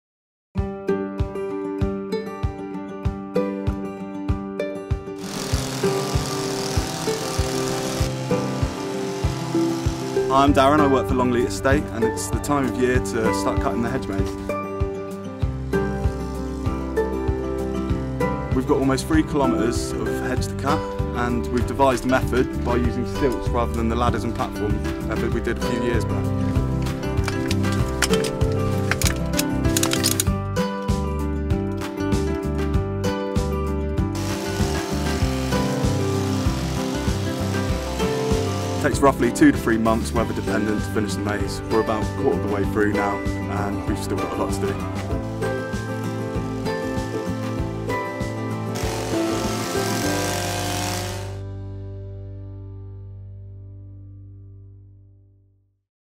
Staff Adopt Cutting Edge Approach for Longleat Maze Trim
Gardeners at Longleat in Wiltshire are making use of stilts to dramatically speed up cutting their world-famous hedge maze.

Made up of more than 16,000 English yew trees, with 2.8kms of pathways and covering an area of 0.6 hectares, the Longleat hedge maze is one of the largest in the world.